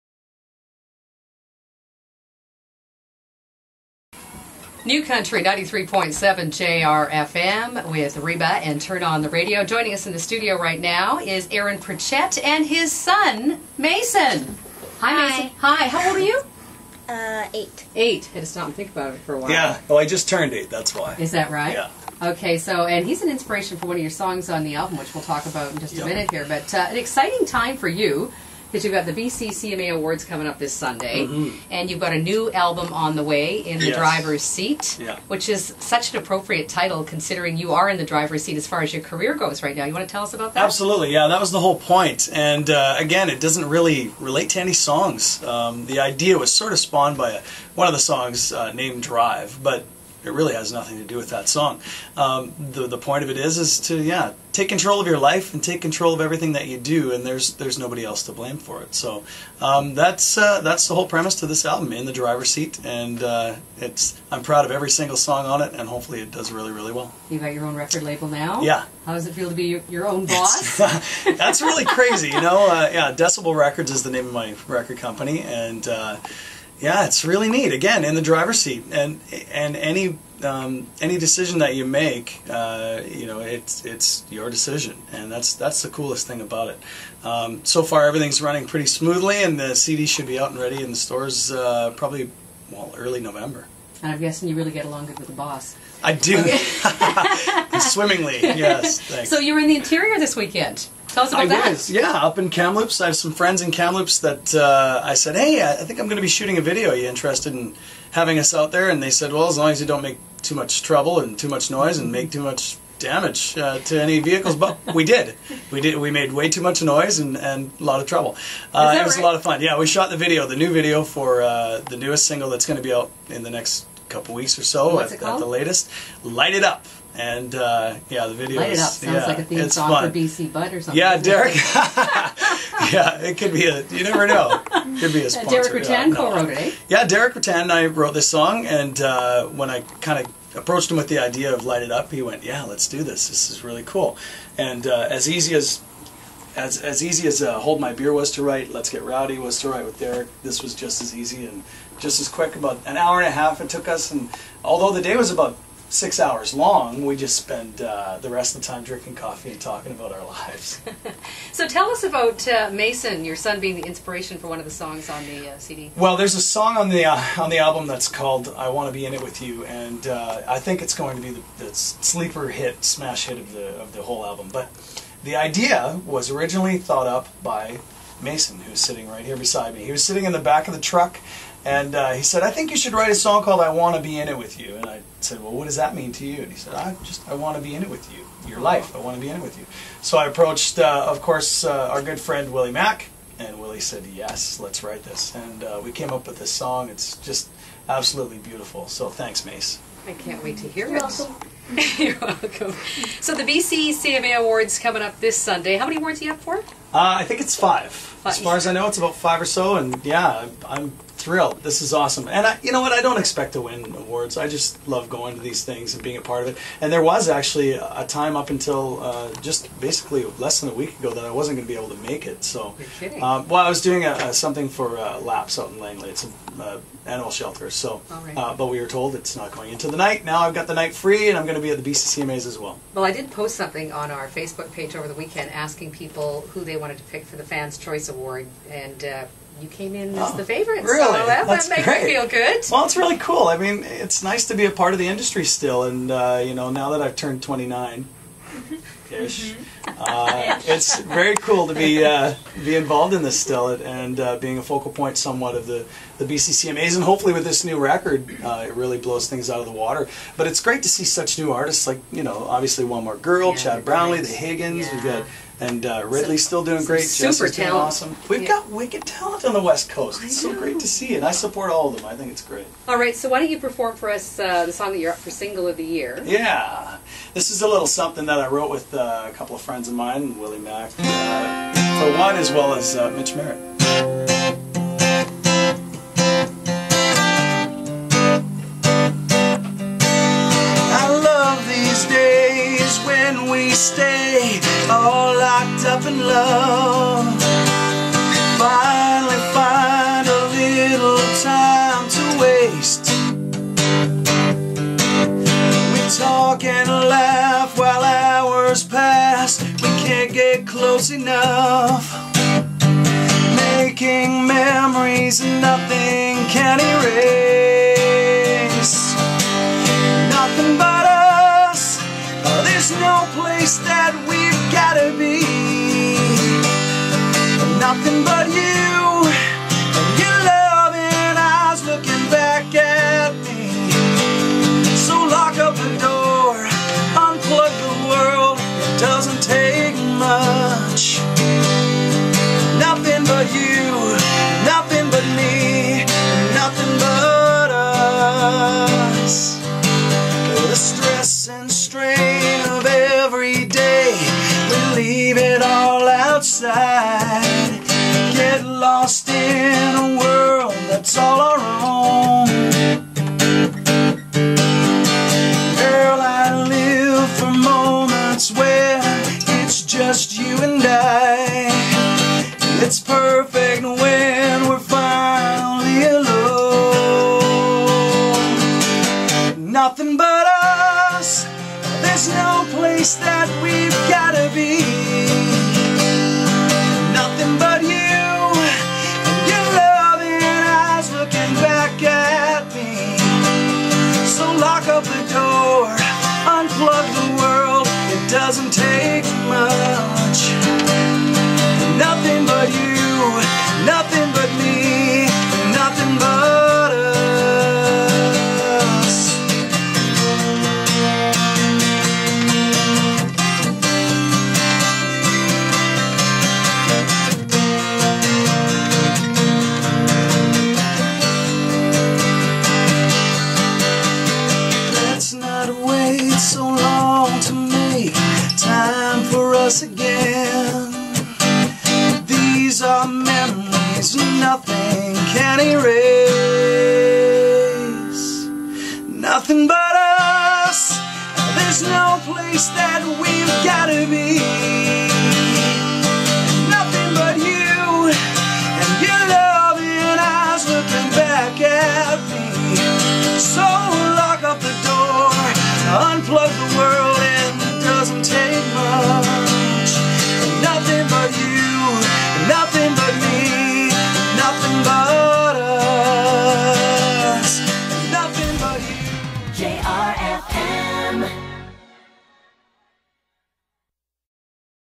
Aaron Pritchett with Colleen Troy
Aaron Pritchett visits 93.7 JRfm and Colleen Troy to talk about his new CD, a fresh new start and the BCCMA Award Show.